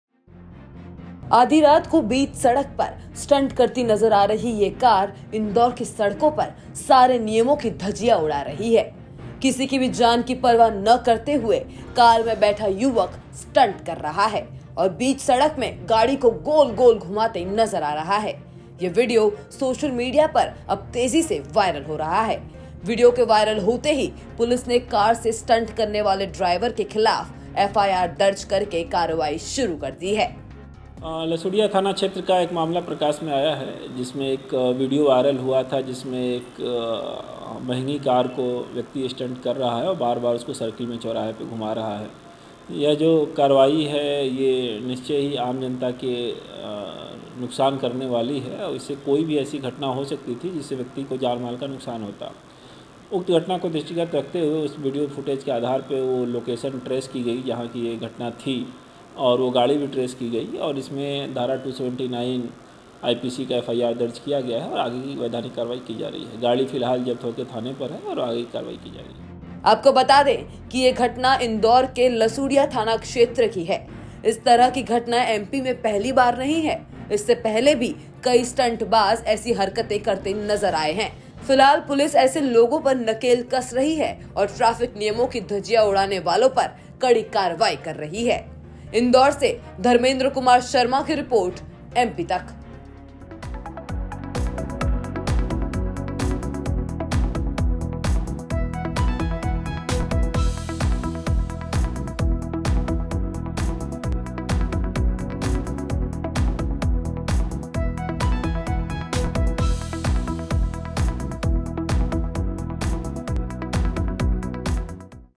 Indore में स्टंटबाजों ने मचाया कहर लेकिन पुलिस ने दबोच कर ये हाल कर दिया ! | MP Tak
Indore में स्टंटबाजी करते हुए कार का वीडियो वायरल. पुलिस ने स्टंट बाज युवक को गिरफ्तार करके दर्ज की FIR. देखिए पूरी रिपोर्ट.